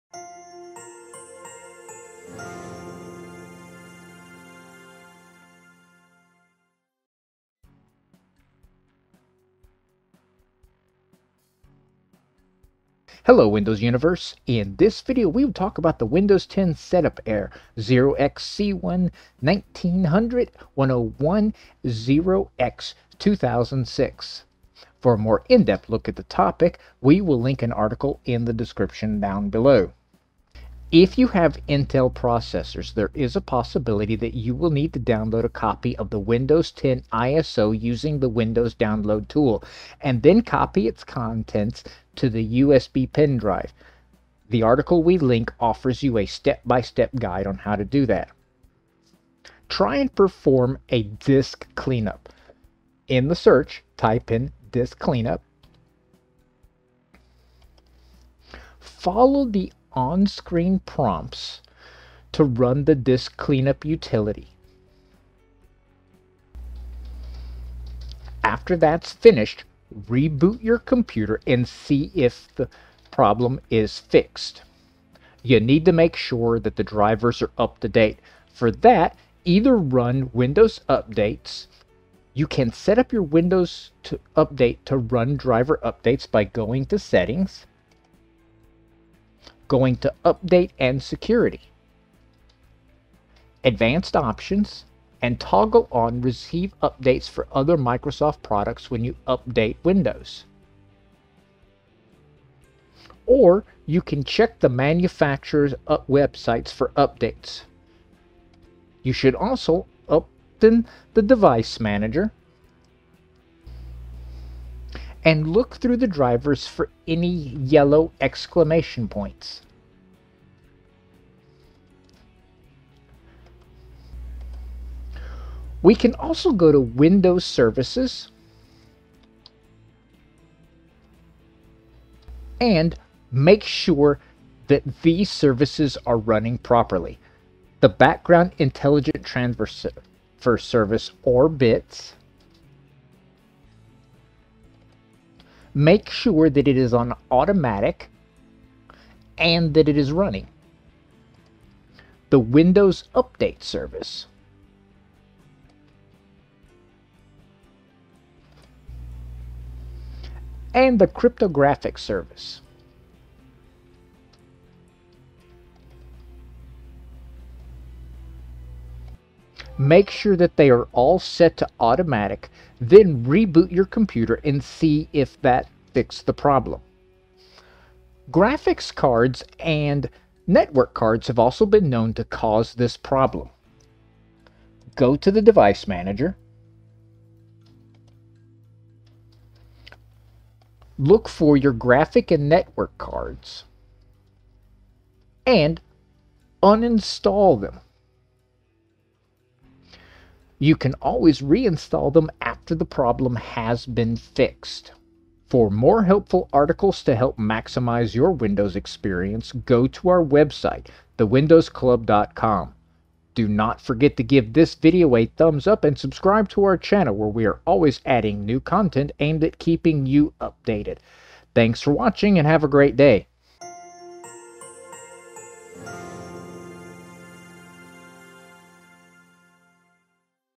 Installation failed in SAFE_OS phase with an error during Replicate_OC operation, 0xC1900101–0x20006
Users have been reporting on the Internet that whenever they try to upgrade Windows 7 or Windows 8.1 to Windows 10, they get an error that says, Error 0xC1900101 – 0x20006, The installation failed in SAFE_OS phase with an error during Replicate_OC operation.

As always, I would recommend creating a System Restore Point just in case you can roll back to the current state if something goes wrong.

1] By changing the Windows 10 installation

2] Perform a Disk Cleanup

3] Make sure that your drivers are up to date

4] Update your system’s BIOS

5] Check the status of Windows Services

6] Checking the RAM Modules